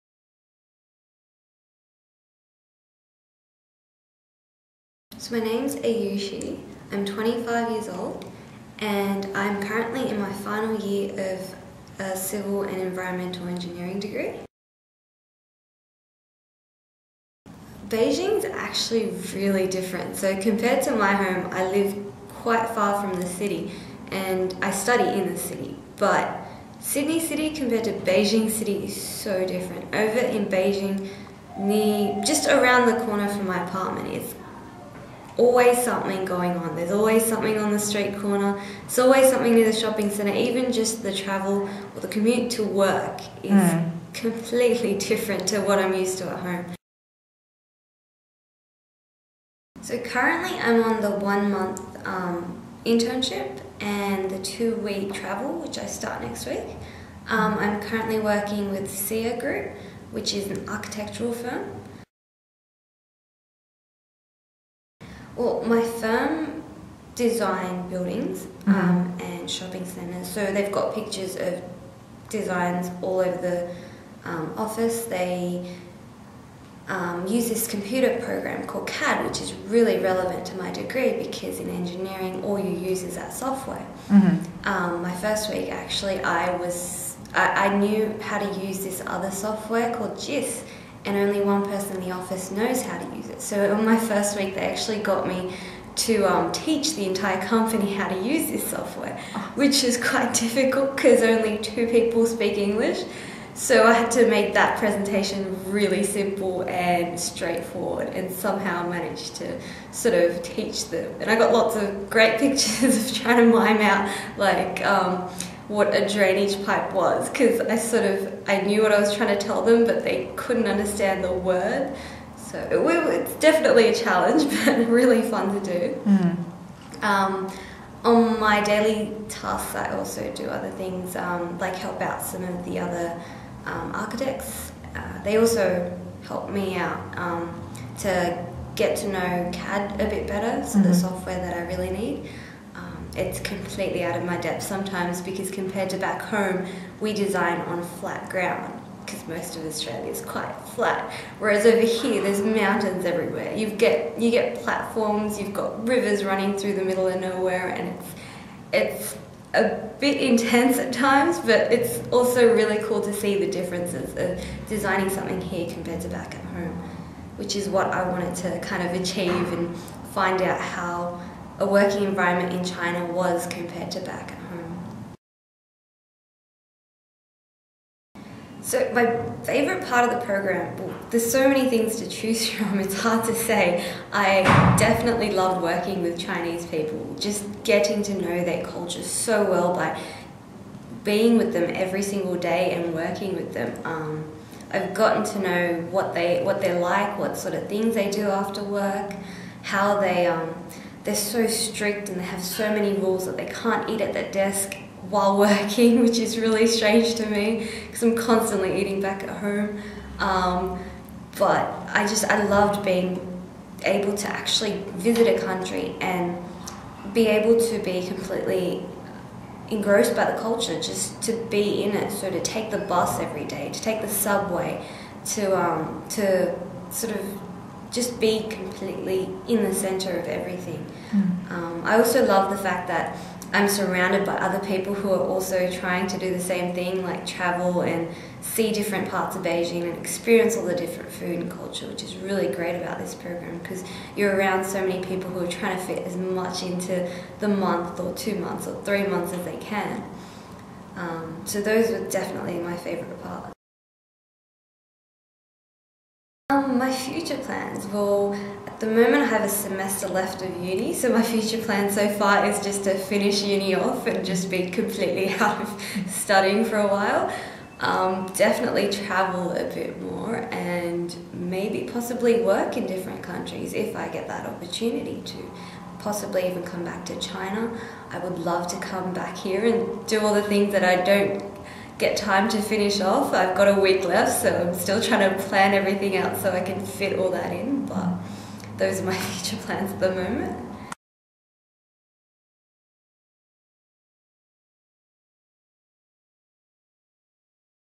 Meet Ayushi - CRCC Asia 2013 Engineering Intern in Beijing
Ayushi Sahay was placed at an Engineering Firm in Beijing for one month in December 2013 before doing the Snow Dragon Trip around China in January. From the University of Technology, Sydney